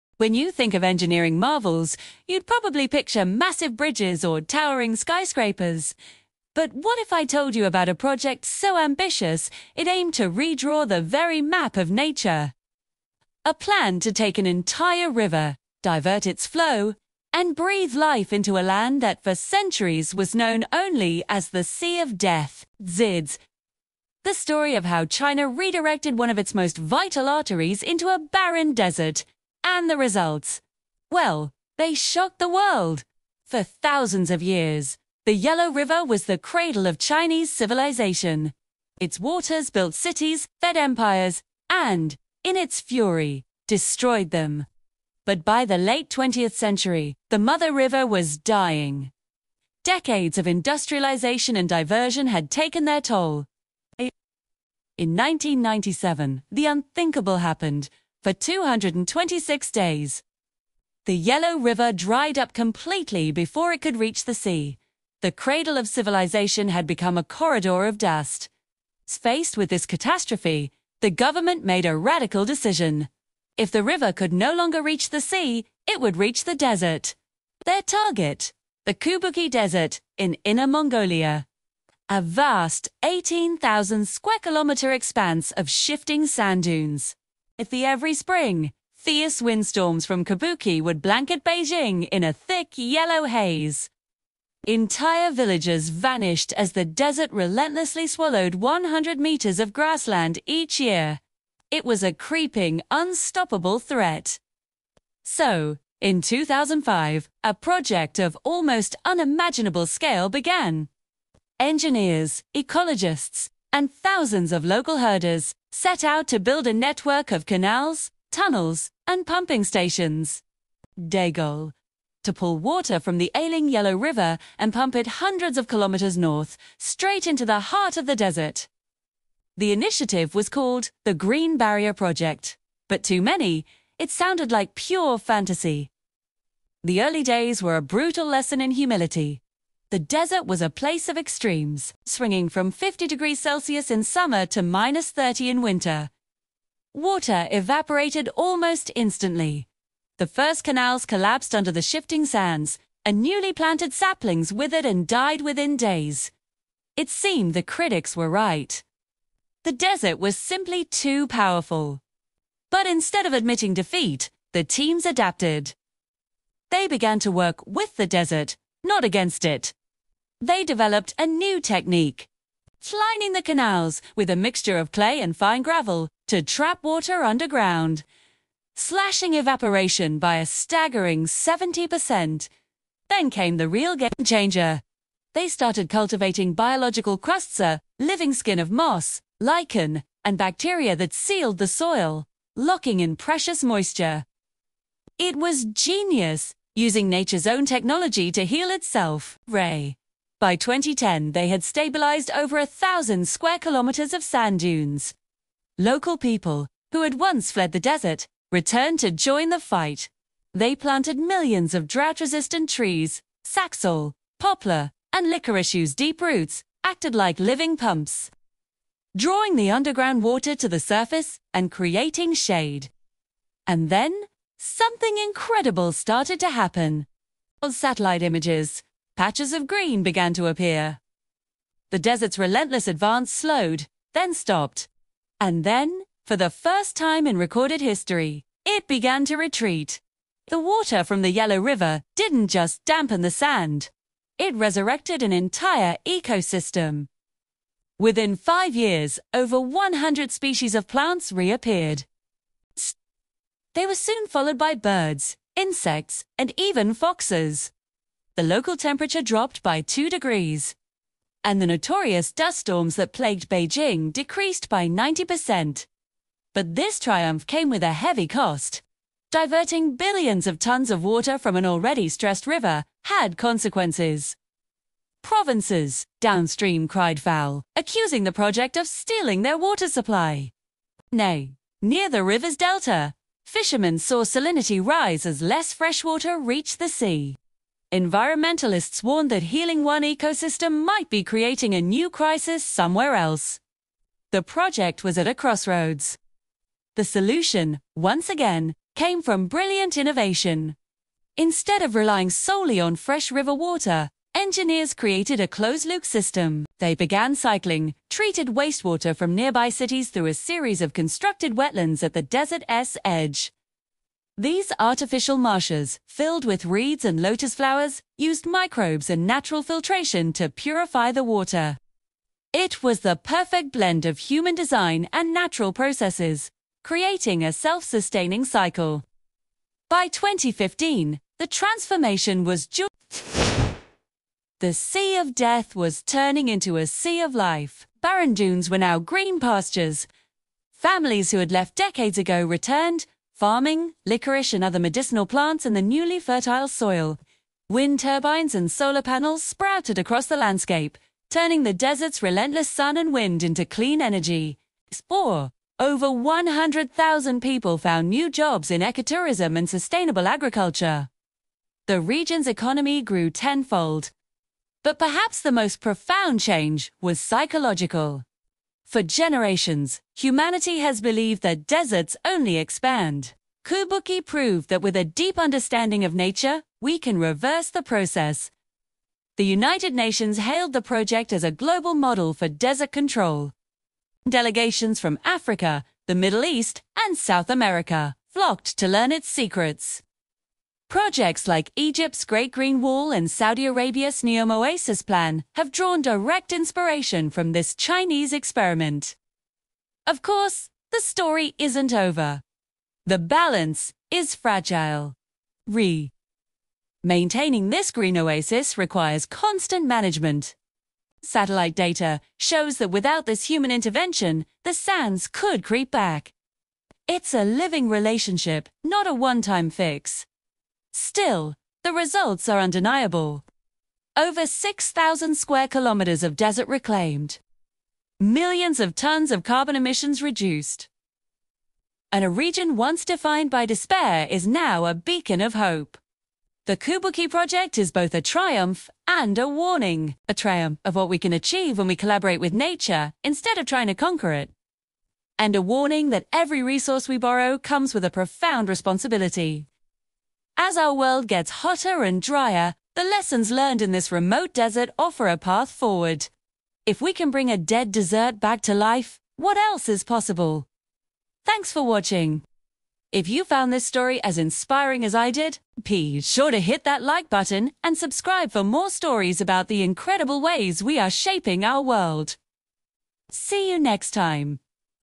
China Redirected an Entire River into Dead Land - The Results Shocked the World
China took on a challenge that defied geography: redirecting the mighty Yellow River to bring life to the barren Kubuqi Desert (The "Sea of Death") in Inner Mongolia. This was a desperate move to save the drying Yellow River and stop massive sandstorms from choking Beijing.

For decades, scientists called this land unsalvageable. But by building hundreds of kilometers of canals and using a groundbreaking natural technique—sealing the soil with biological crusts (moss and bacteria)—engineers achieved the unthinkable.

This short documentary explores how they shrank the desert for the first time in recorded history, revived over 6,000 square kilometers of dead land, and brought back plant and animal life.

Discover the triumph and the cost of this massive undertaking, which transformed Kubuqi into a global model of circular economy, powered by solar energy and sustainable farming. It's not just a story of engineering; it's a testament to humanity's ability to collaborate with nature and turn a "Sea of Death" into a vibrant, living ecosystem.